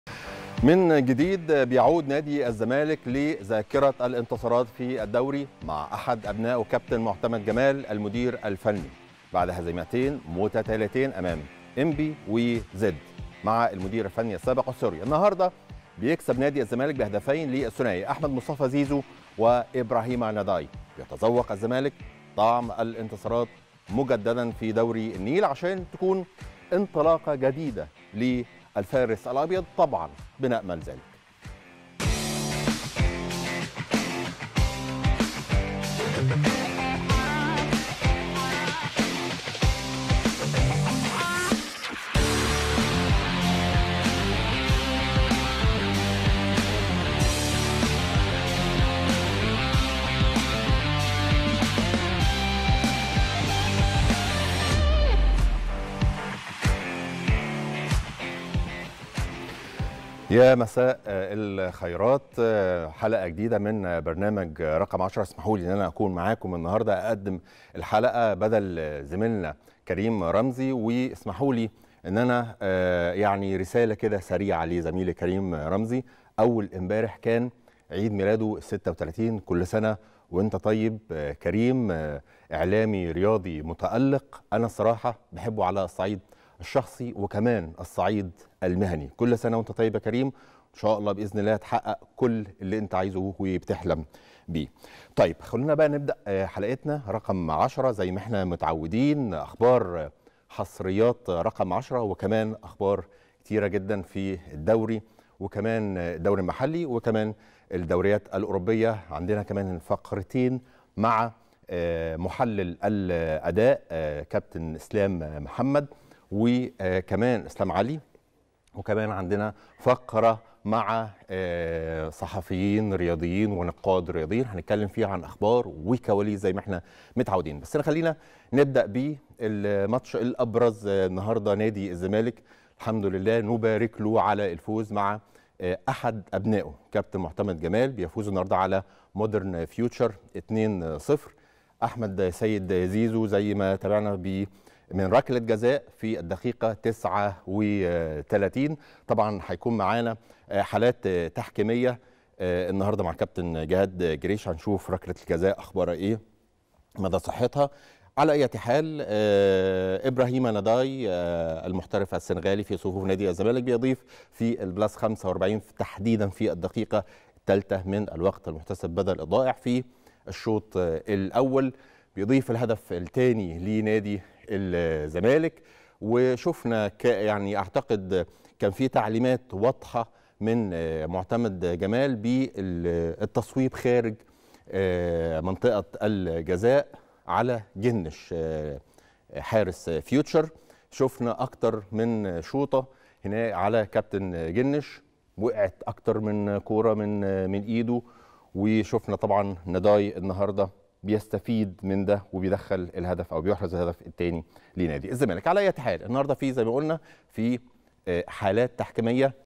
“ الزمالك يعود من جديد لإدارة الانتصارات “🔥🔥 مقدمة قوية من رجائي رمزي بعد فوز الزمالك على فيوتشر 💣👇🏻
“ الزمالك يعود من جديد لإدارة الانتصارات “🔥🔥 .. مقدمة قوية من رجائي رمزي بعد فوز الزمالك على مودرن فيوتشر بثنائية نظيفة 💣👇🏻